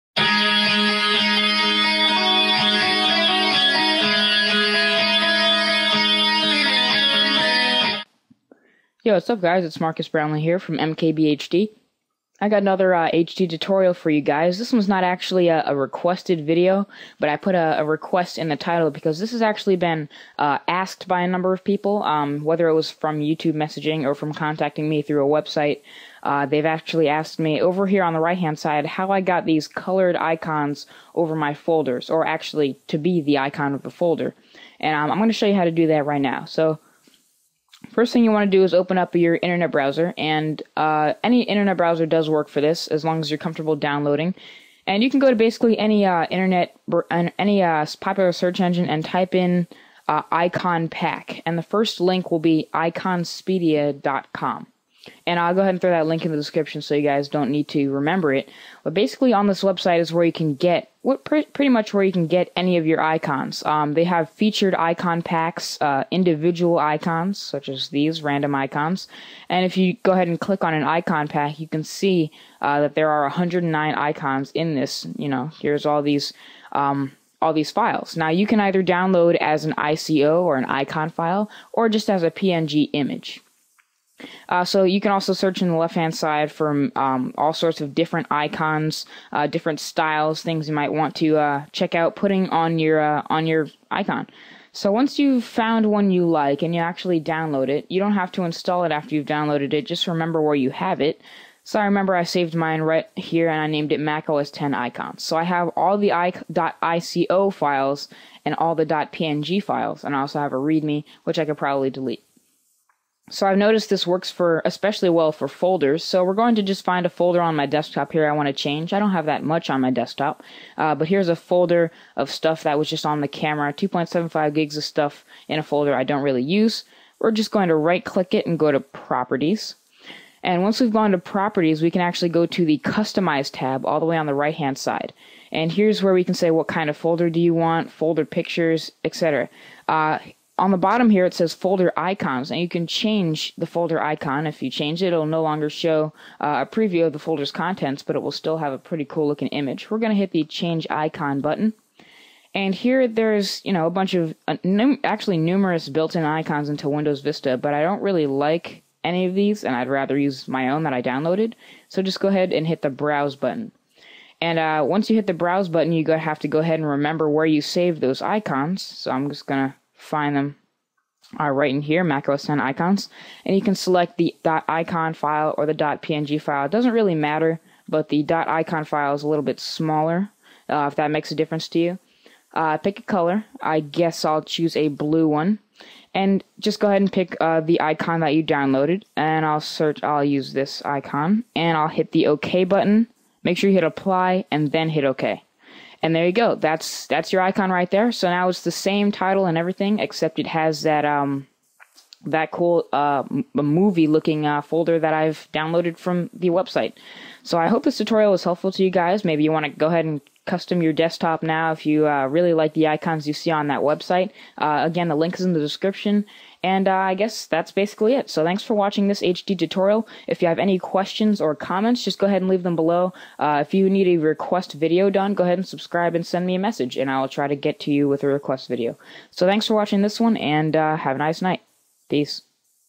HD Tutorial: Custom Desktop Icons (Request)
In this tutorial, I'll give a rundown of how and where to get and apply custom downloaded icons to your desktop or your individual folders. This also works for Rocket Dock and Object Dock icons.

Note: This works for folders, but not (as far as I know) for .exe files and others.